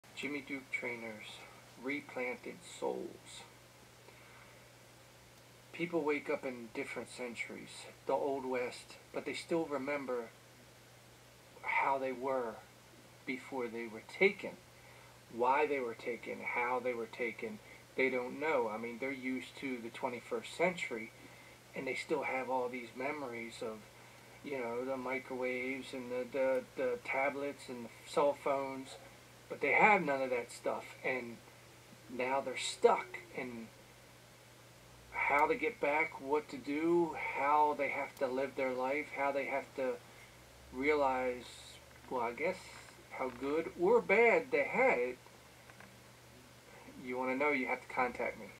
REplanted Souls
People wake up in different century. The old west but still have there 21 century memories.

New story ideas by Jimmy Duke Traynor
"Traynor4films"